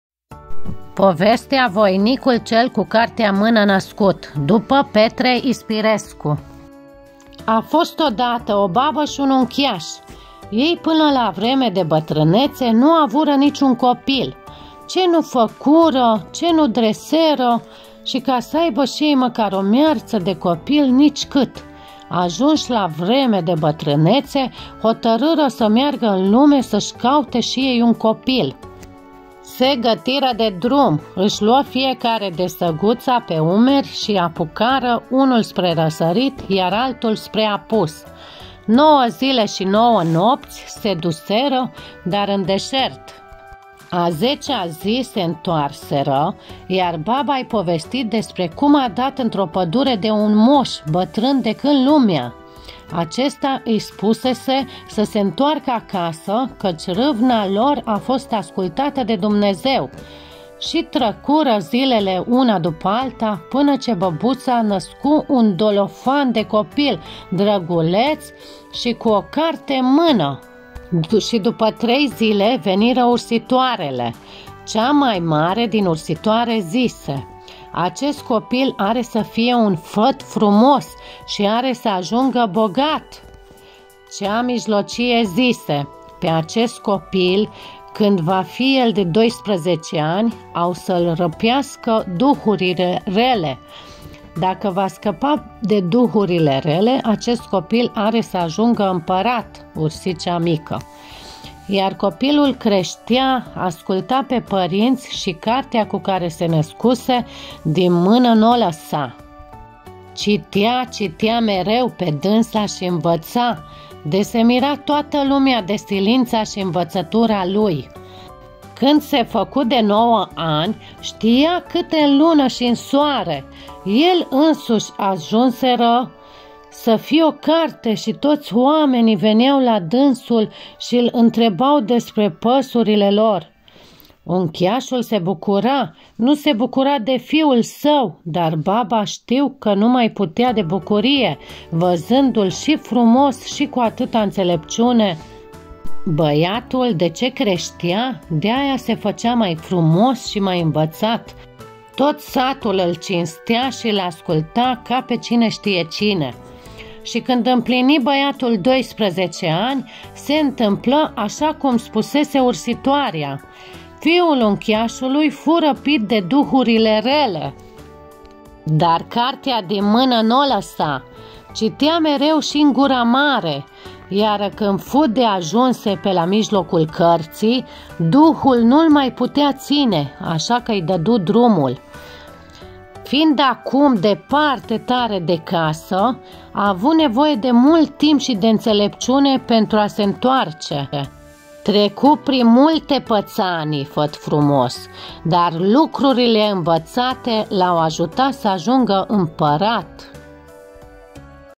Povestea "Voinicul cel cu cartea în mână născut" 🤴 după Petre Ispirescu
Povestea este din manualul de limba
română din clasa a-III-a.